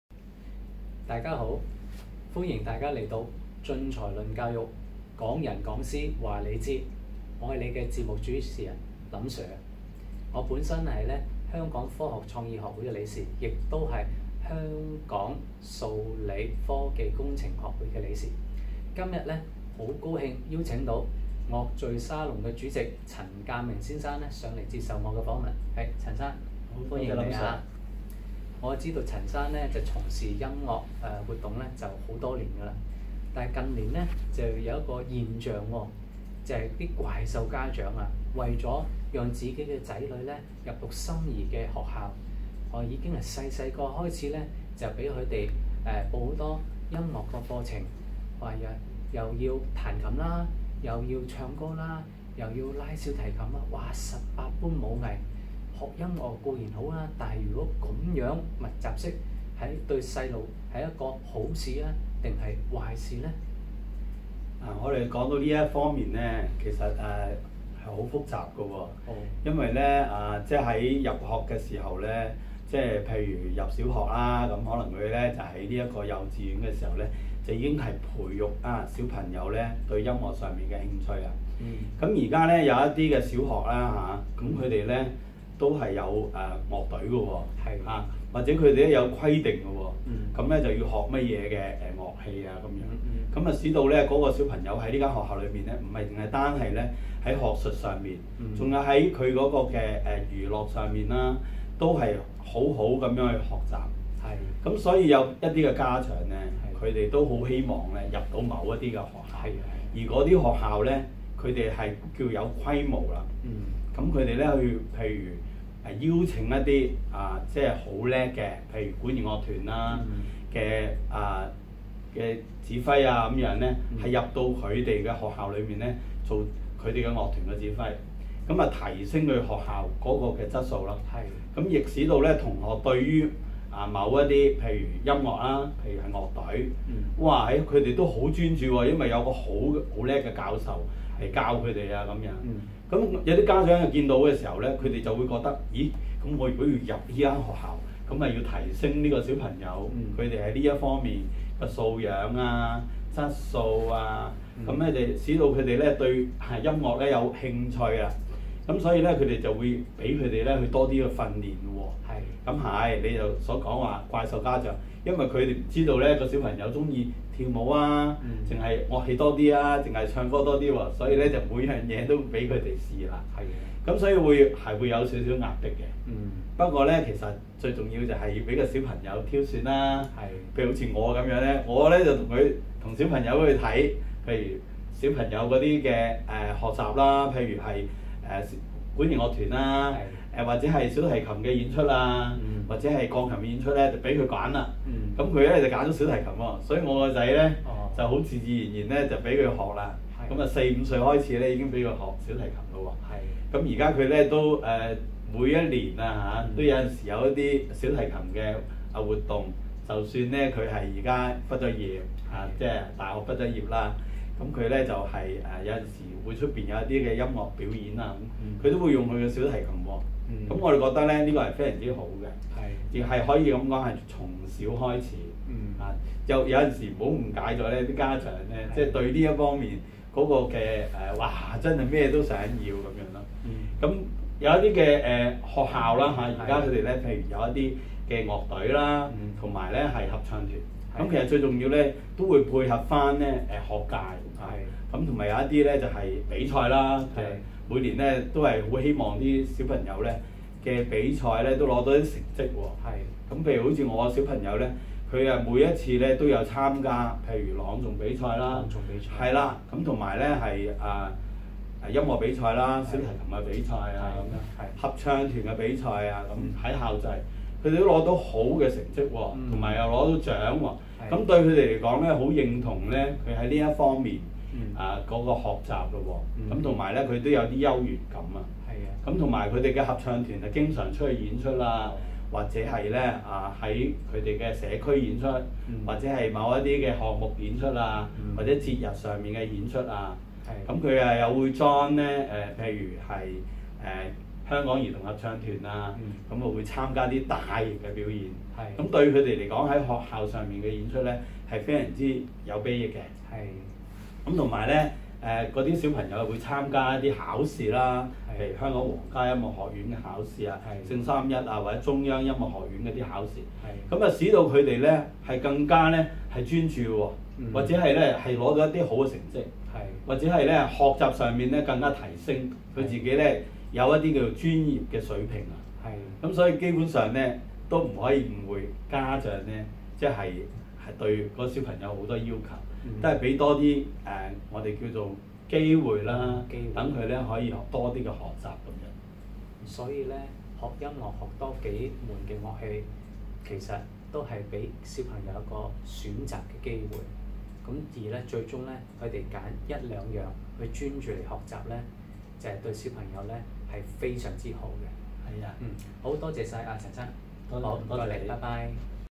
第四集主題：現時許多家長要求子女學習音樂，以便報讀心怡學校。你如何看這現象？
晉才論教育 - 港人講師話你知
本會附屬機構 - 晉才教育會不定期邀請各教育界專家於網上分享教育現況與發展！

主持︰林伯強先生 (香港科學創意學會理事、香港數理科技工程學會理事)
嘉賓︰陳鑑明先生 (樂敘沙龍主席、康宏理財服務有限公司聯席董事)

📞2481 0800
Address🚪：九龍旺角登打士街23號嘉興商業中心17樓06室